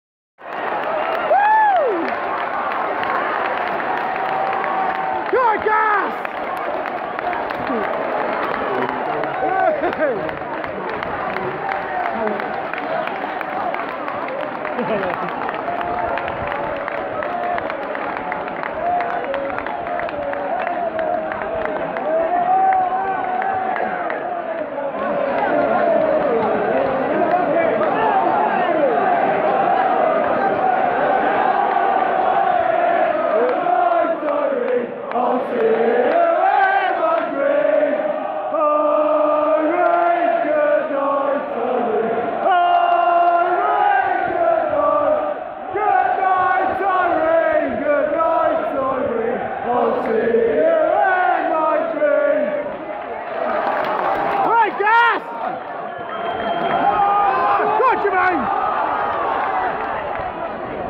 Bristol rovers vs yeovil after harrison goal! UTG
Celebrations after harrison goal against yeovil